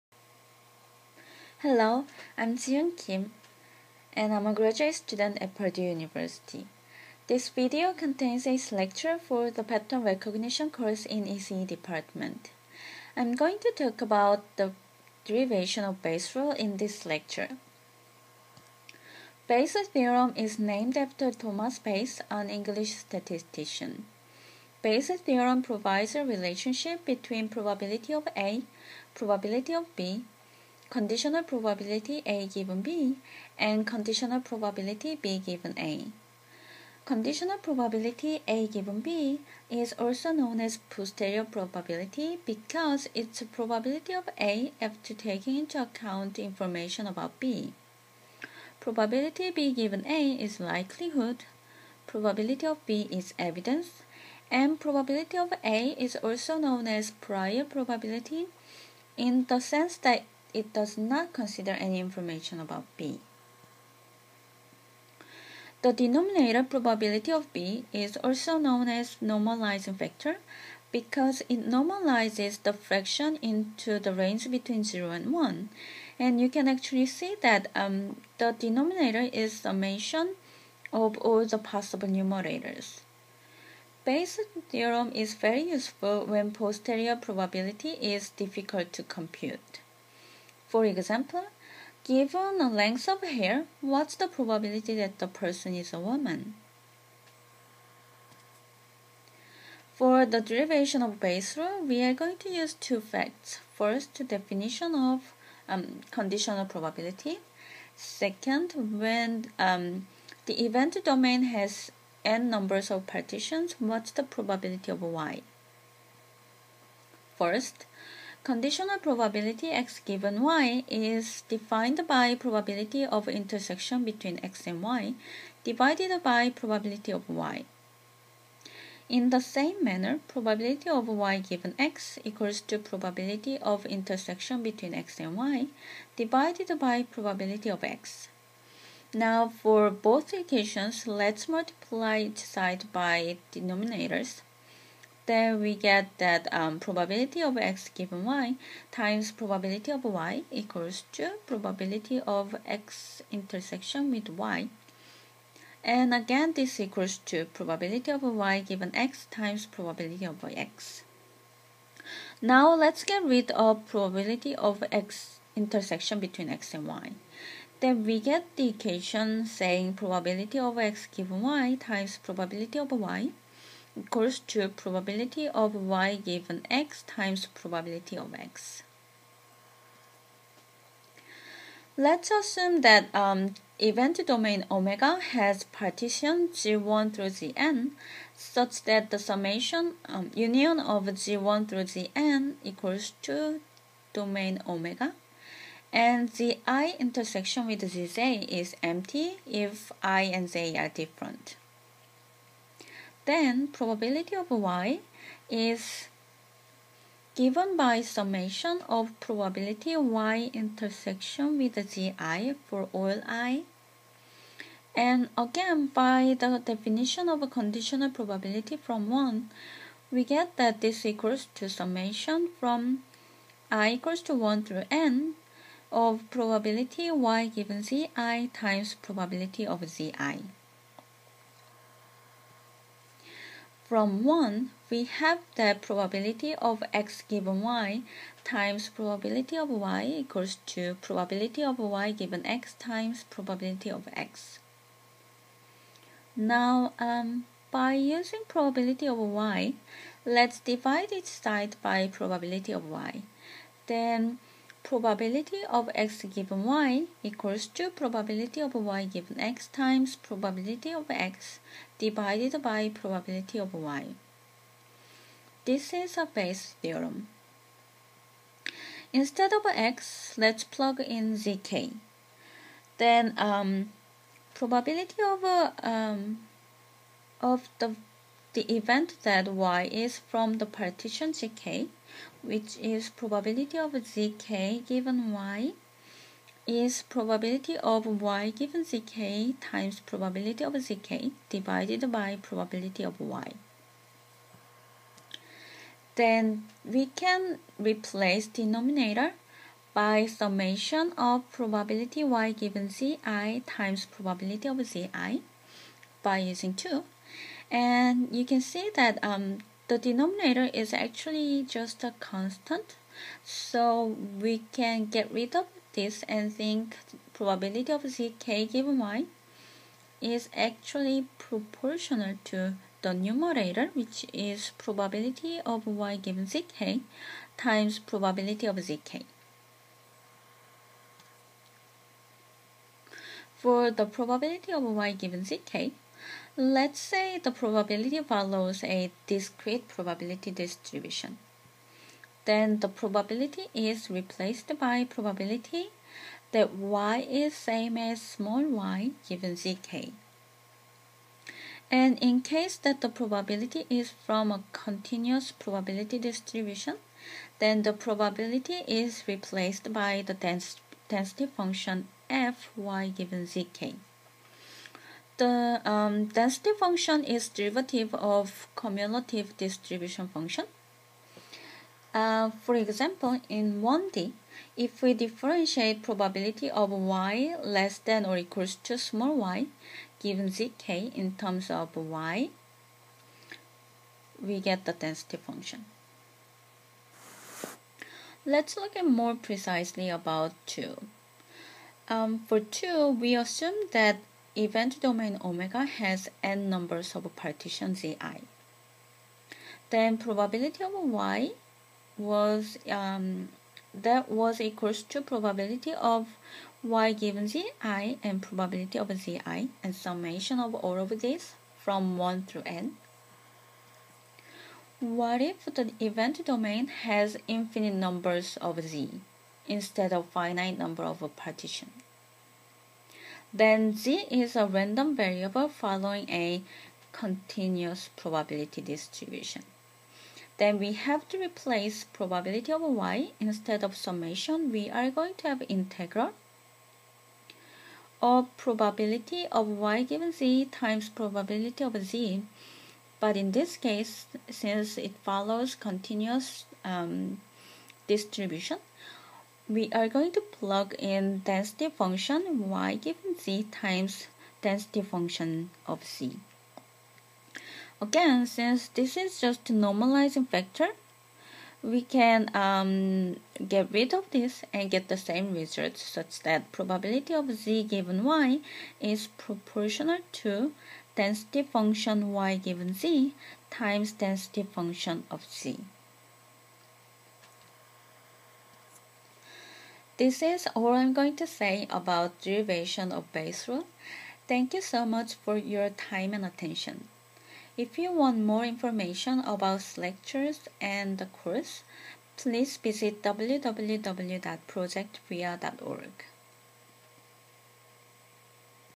Derivation of Bayes' Rule, a slecture by Jieun Kim
This is a slecture for Prof. Boutin's course on Statistical Pattern Recognition (ECE662) made by Purdue Math Ph.D. student Jieun Kim. The complete slecture is posted at

To view other slectures on the same topic in the English language, go to the ECE662 course wiki